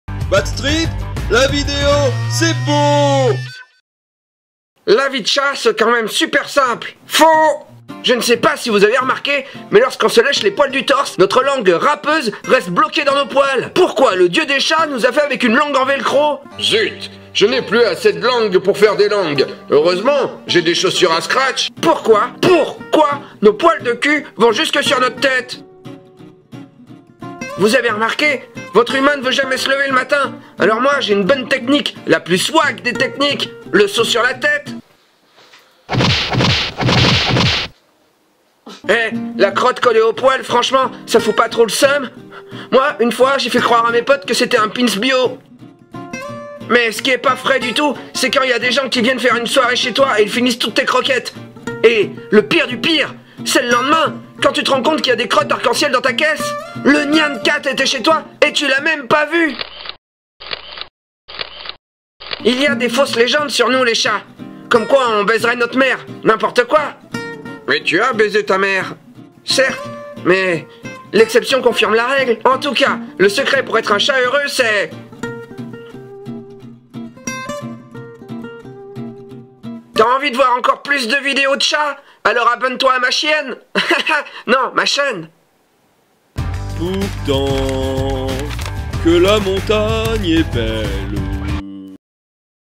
DAVY - PODCAT
Les chats aussi peuvent faire des Podcast. Voici le PODCAT.